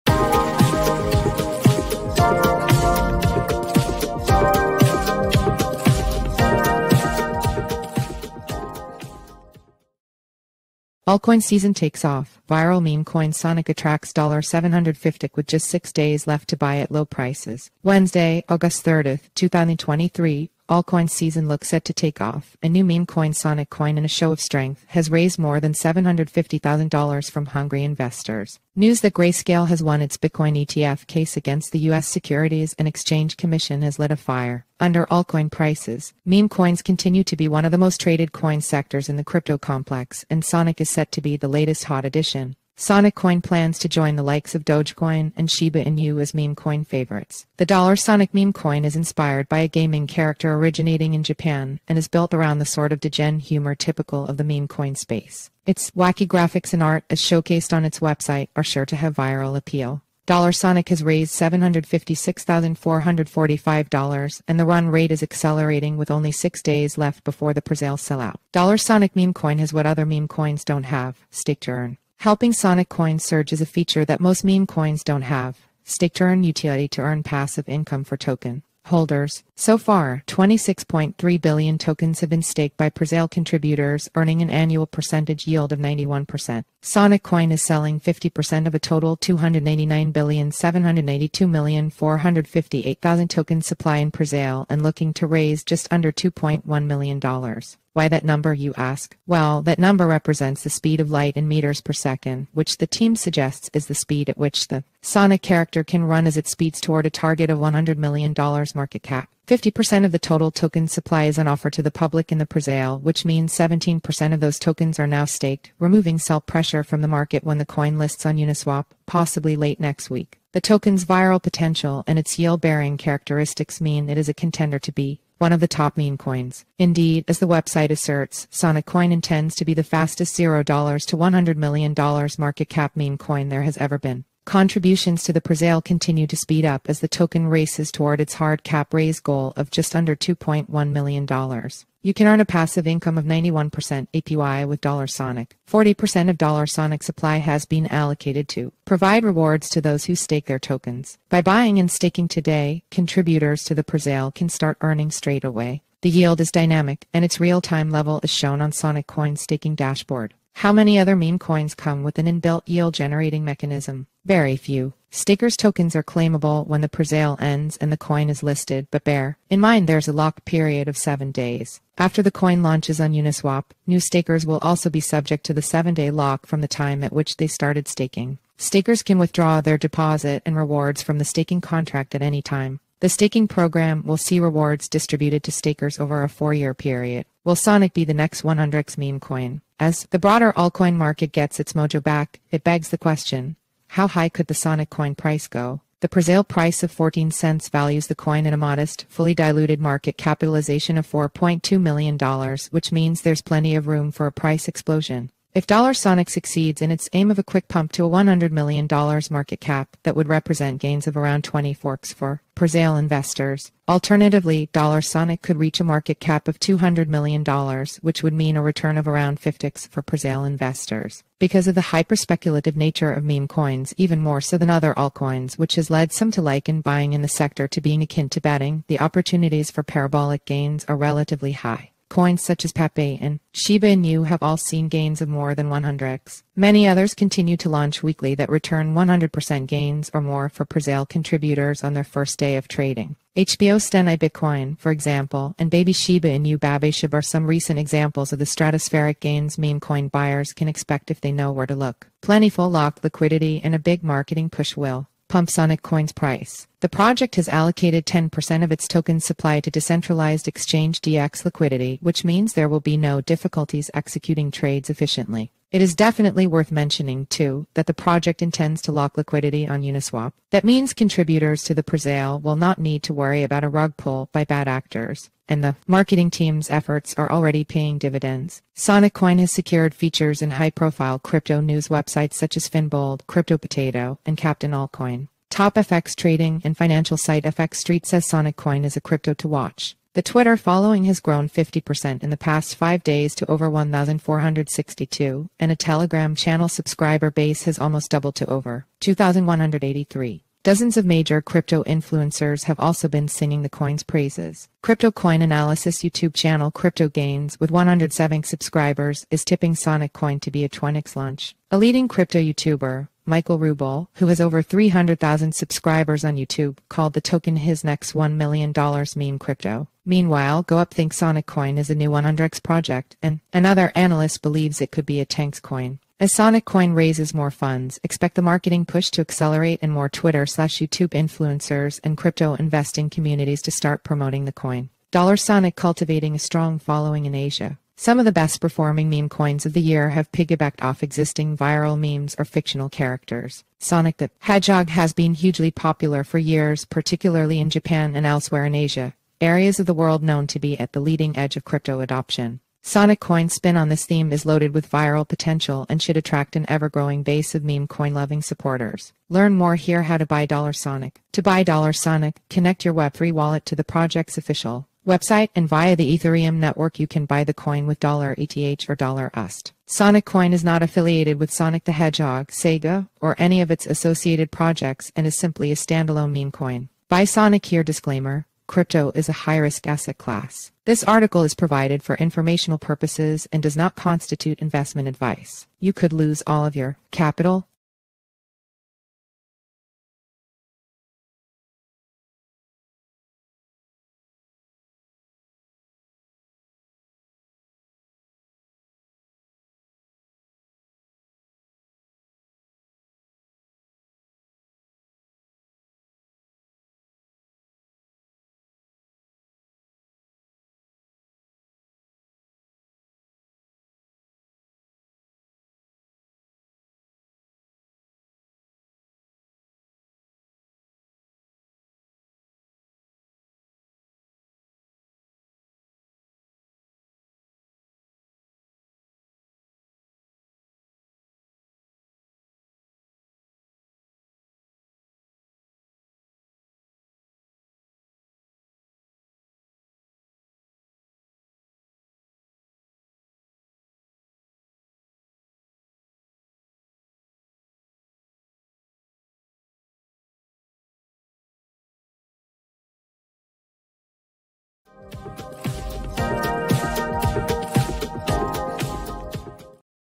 Altcoin Season Takes Off, Viral Meme Coin Sonik Attracts $750k With Just 6 Days Left to Buy at Low
Wednesday, August 30th, 2023 – Altcoin season looks set to take off, and new meme coin Sonik Coin in a show of strength, has raised more than $750,000 from hungry investors.

News that Grayscale has won its bitcoin ETF case against the US Securities and Exchange Commission has lit a fire under altcoin prices.

Meme coins continue to be one of the most traded coin sectors in the crypto complex, and Sonik is set to be the latest hot addition.

Sonik Coin plans to join the likes of Dogecoin and Shiba Inu as meme coin favorites.

The $SONIK meme coin is inspired by a gaming character originating in Japan and is built around the sort of degen humor typical of the meme coin space. Its wacky graphics and art, as showcased on its website, are sure to have viral appeal.

$SONIK has raised $756,445, and the run rate is accelerating with only six days left before the presale sell-out.

$SONIK meme coin has what other meme coins don’t have – stake-to-earn.

Helping Sonik Coin’s surge is a feature that most meme coins don’t have –  stake-to-earn utility to earn passive income for token holders.

So far, 26.3 billion tokens have been staked by presale contributors, earning an annual percentage yield of 91%.

Sonik Coin is selling 50% of a total 299,792,458,000 token supply in presale and looking to raise just under $2.1 million.

Why that number, you ask?

Well, that number represents the speed of light in meters per second, which the team suggests is the speed at which the Sonik character can run as it speeds toward a target of $100 million market cap.

Fifty percent of the total token supply is on offer to the public in the presale, which means 17% of those tokens are now staked, removing sell pressure from the market when the coin lists on Uniswap, possibly late next week.

The token’s viral potential and its yield-bearing characteristics mean it is a contender to be one of the top meme coins. Indeed, as the website asserts, Sonik Coin intends to be the fastest $0 to $100 million market cap meme coin there has ever been.

Contributions to the presale continue to speed up as the token races toward its hard cap raise goal of just under $2.1 million.

You can earn a passive income of 91% APY with $SONIK.

Forty percent of $SONIK’s supply has been allocated to provide rewards to those who stake their tokens.

By buying and staking today, contributors to the presale can start earning straight away. The yield is dynamic, and its real-time level is shown on Sonik Coin’s staking dashboard.

How many other meme coins come with an in-built yield-generating mechanism? Very few.

Stakers’ tokens are claimable when the presale ends and the coin is listed, but bear in mind there’s a lock period of 7 days.

After the coin launches on Uniswap, new stakers will also be subject to the seven-day lock from the time at which they started staking. Stakers can withdraw their deposit and rewards from the staking contract at any time.

The staking program will see rewards distributed to stakers over a four-year period.

Will Sonik be the next 100x meme coin?.

As the broader altcoin market gets its mojo back, it begs the question: how high could the Sonik coin price go?

The presale price of $0.000014 values the coin at a modest, fully diluted market capitalization of $4.2 million, which means there’s plenty of room for a price explosion.

If $SONIK succeeds...